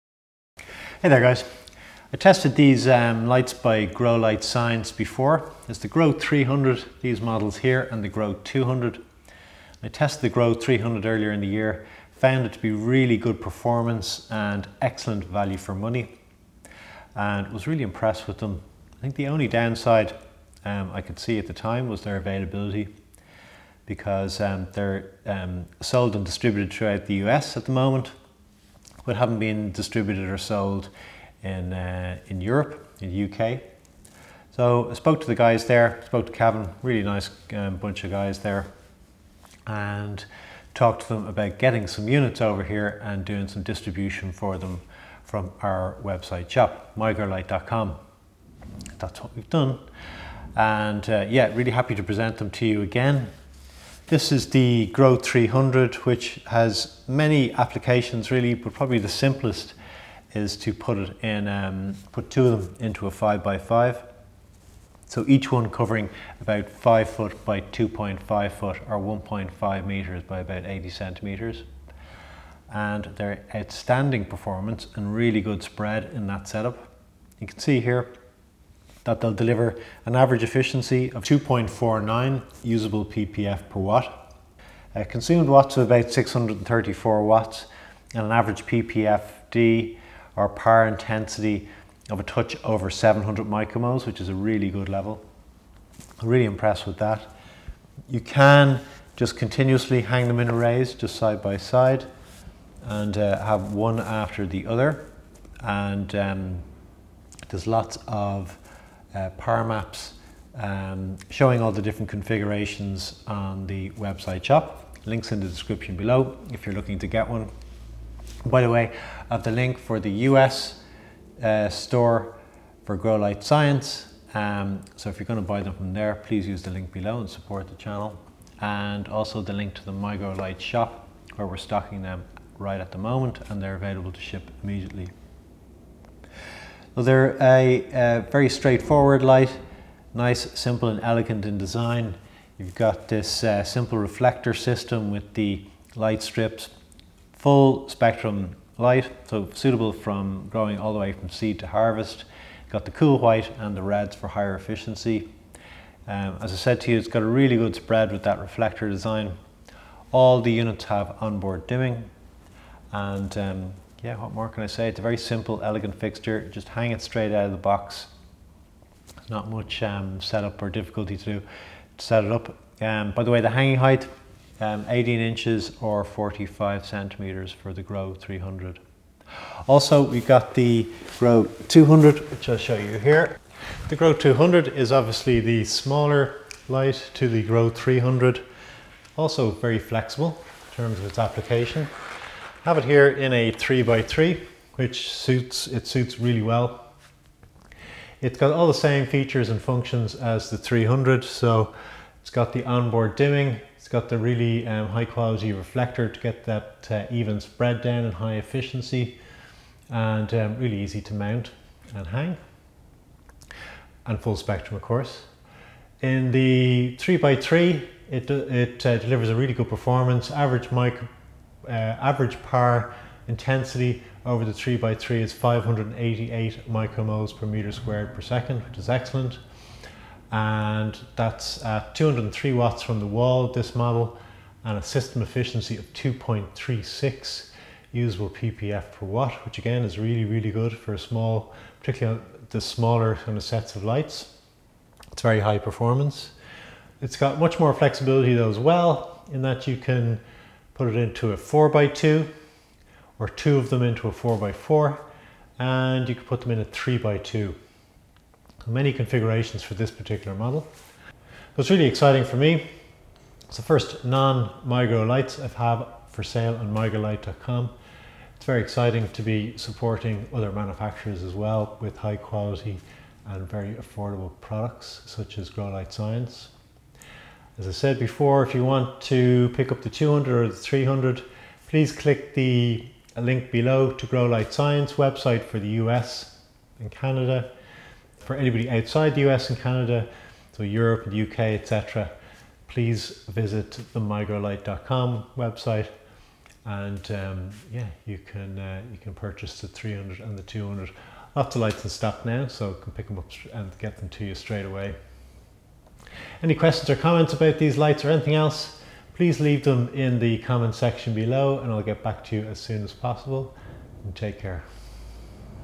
Grow 300 and grow 200 by Grow light science | Now for sale in Europe and the UK on migrolight.com
The Grow 200 and Grow 300 LED grow lights by Grow Light Science are excellent quality, performance and especially good value for money.

Previously unavailable outside the US and Canada so we have partnered with Grow Light Science to make them available to European and UK growers.

The Grow 200 is suitable for 3 x 3 (90cm x 90cm), 3 x 2 (90cm x 60cm) & 4 x 2 (120cm x 60cm) grow areas or tents.
The Grow 300 is suited for a 5ft x 2.5ft (1.5m x 0.75m) space and two fit very nicely into a 5ft x 5ft (1.5m x1.5m) grow tent.
Full PAR maps for these arrangements are in the product pages.